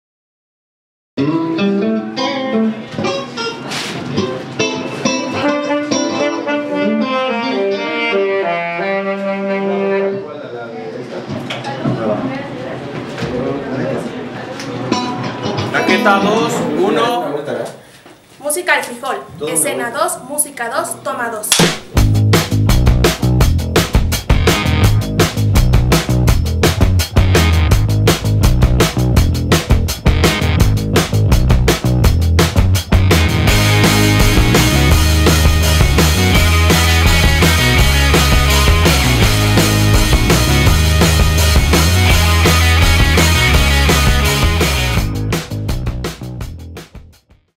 Tráiler 2: Música Al Frijol
Este es un avance de todo lo que MúsicaAlFrijol tiene para ti.
Música independiente, bandas y solistas de la ciudad de Pachuca.